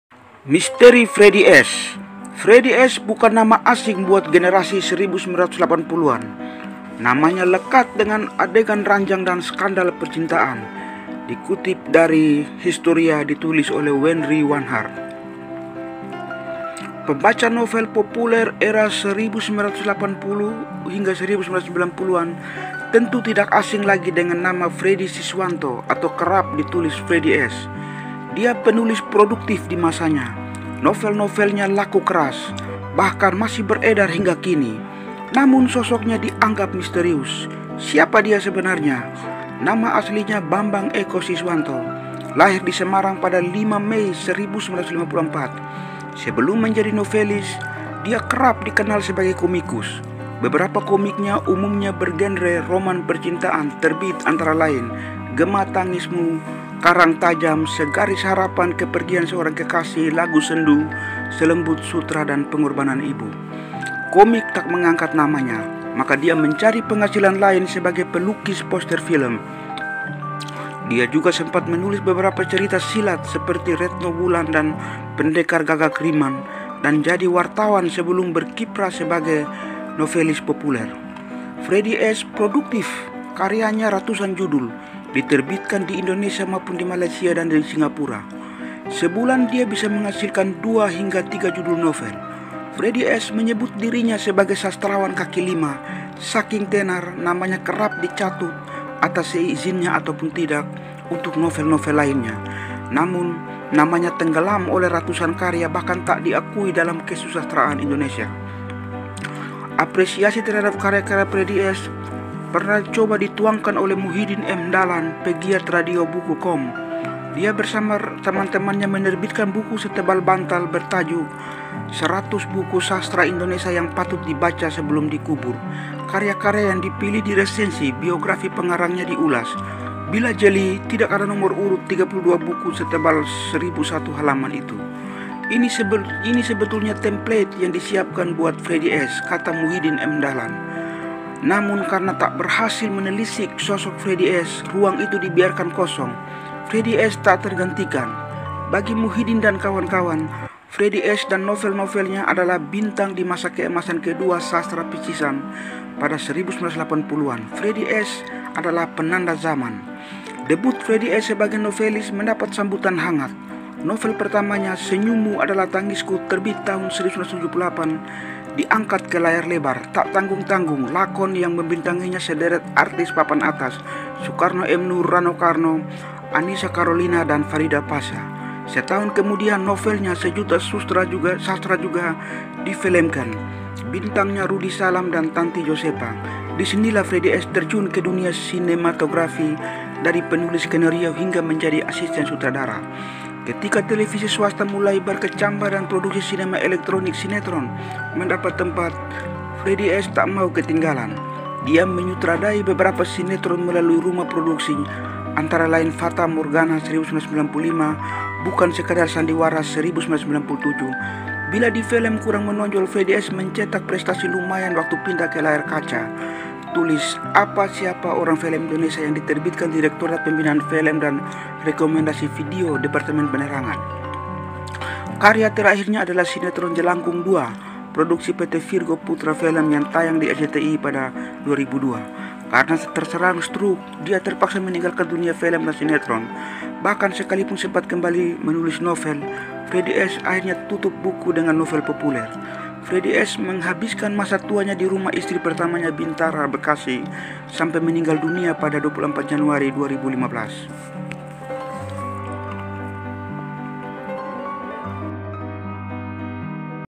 Fredy S. dan karya novel romansa Indonesia
PEMBACA novel romansa Indonesia populer era 1980 hingga 1990-an tentu tak asing lagi dengan nama Fredy Siswanto atau kerap ditulis Fredy S..